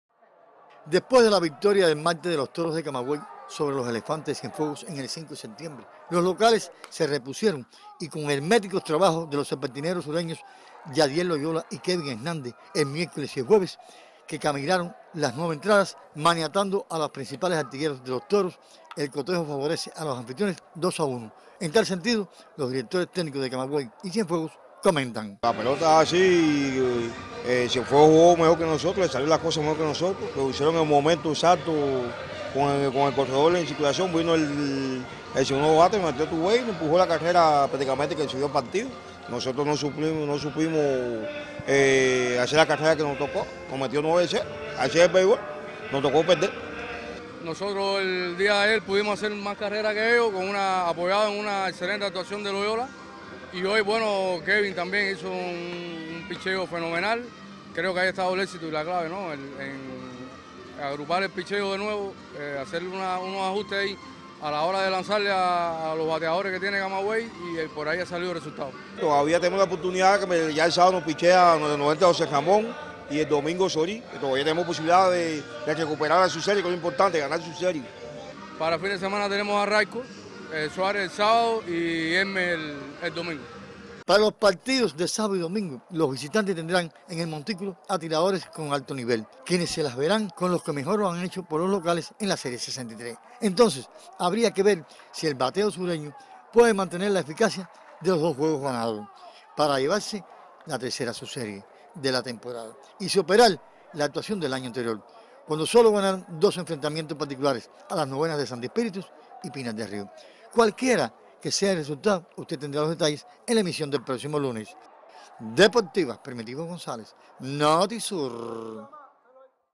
Mejoran su paso Elefantes de Cienfuegos
En el tercer desafío del primer tercio de la Serie Nacional de Béisbol los Elefantes de Cienfuegos ganaron 2 a uno a los Toros de Camagüey.